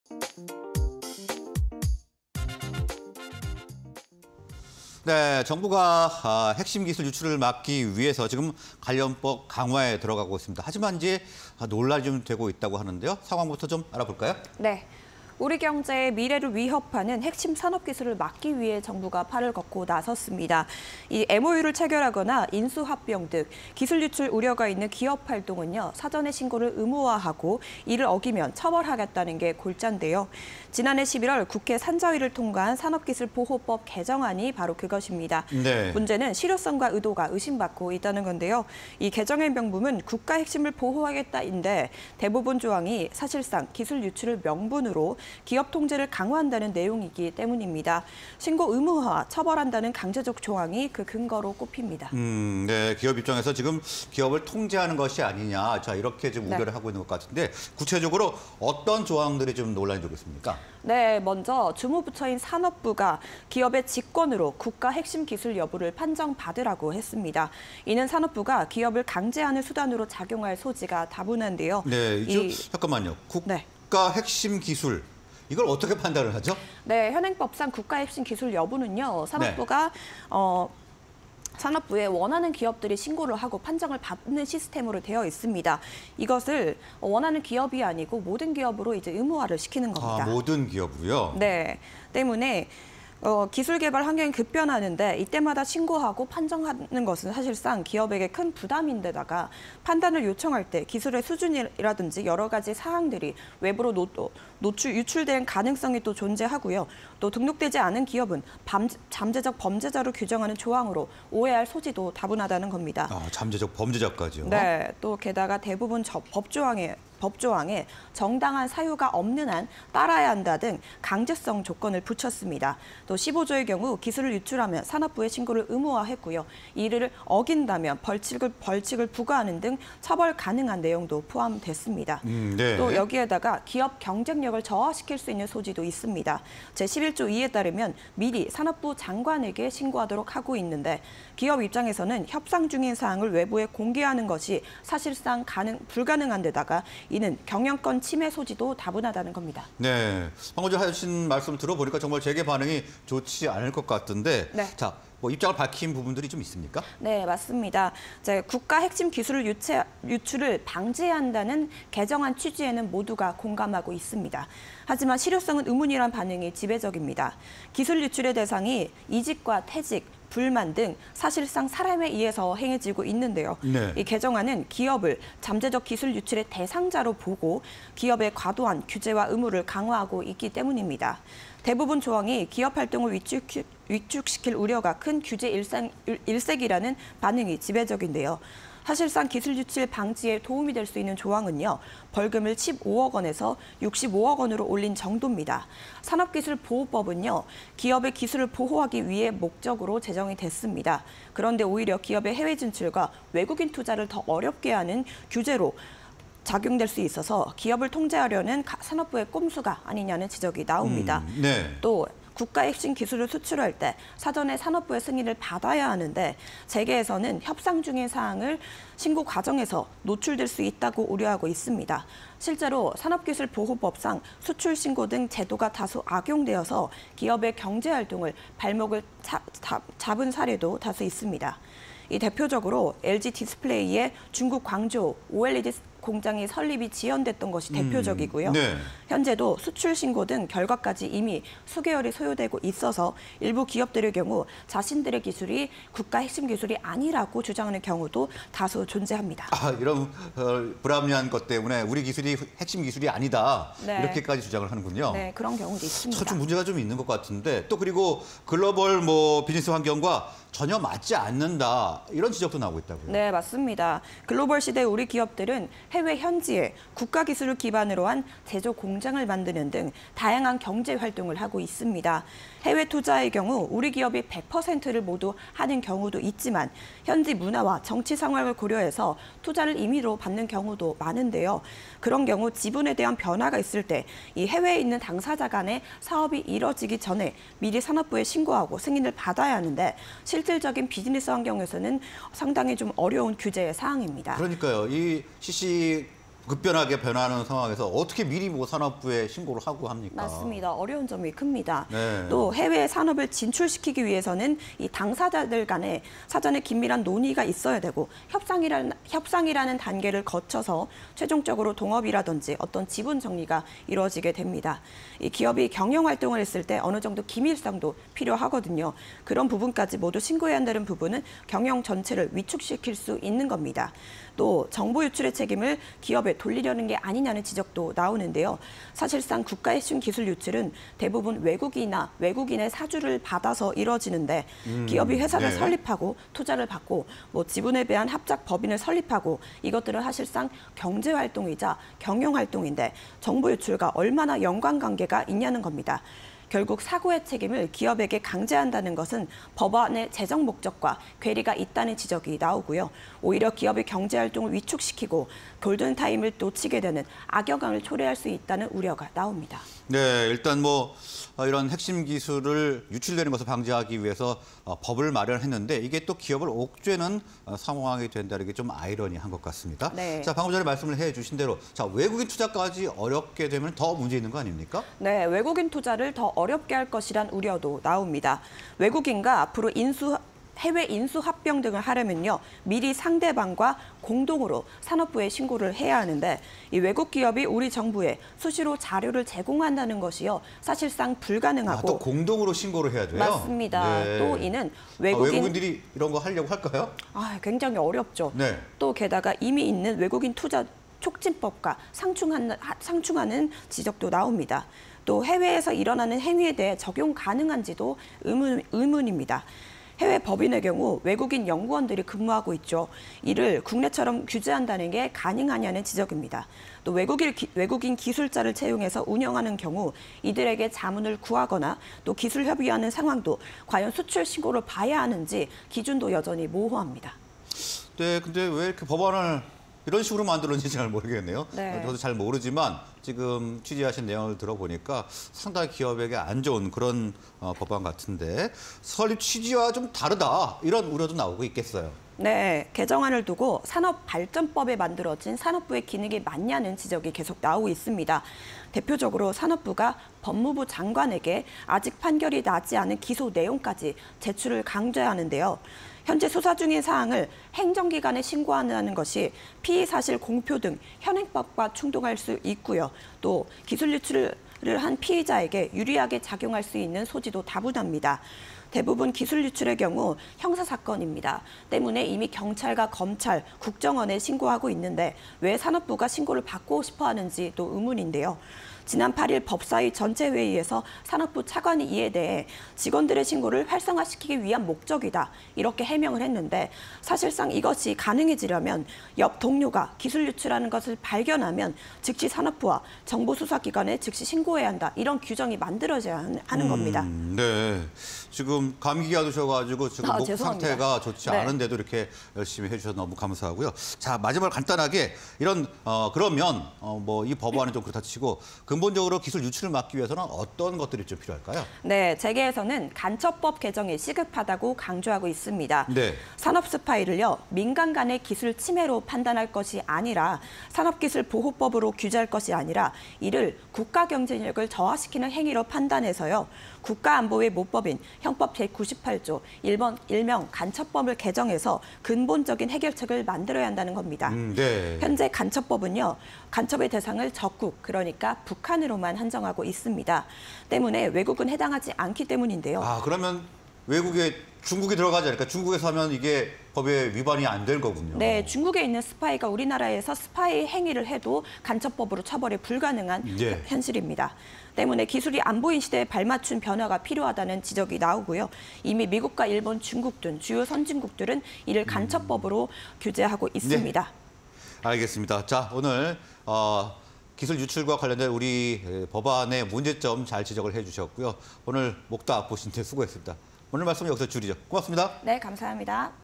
산업스파이 막으라니까... 기업활동 통제하겠다는 산업부_기자들의 백브리핑 (20240112)
Q. 산업기술보호법 개정안 두고 '갑론을박'
Q. 말많은 산업기술보호법, 논란 사항은?
Q. 산업기술보호법, 기업에 독소조항?... 재계 입장은?
Q. 산업기술보호법 개정안, 오히려 해외 진출에 걸림돌?
Q. 산업기술보호법 개정안, 외국인 투자 위축 우려도
Q. 산업기술보호법, 산업부 설립 취지와 다르다?
Q. 재계, 간첩법 개정 통한 근본적인 해결 촉구